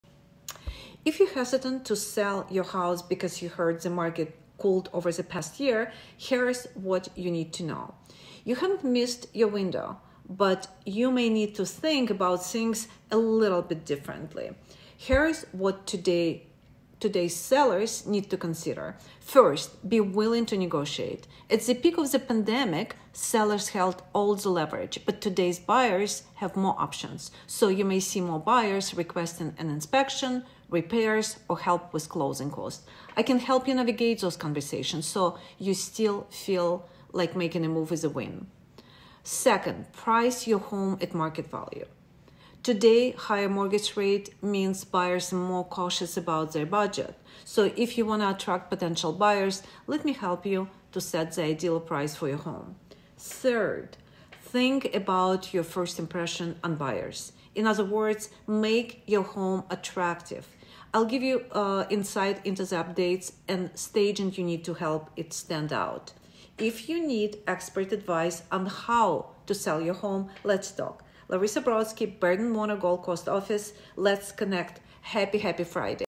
Selling Your House? - Here's What You Need To Know
Real estate Market . Selling your house
3 tips for each homeowner to consider when listing the house for sale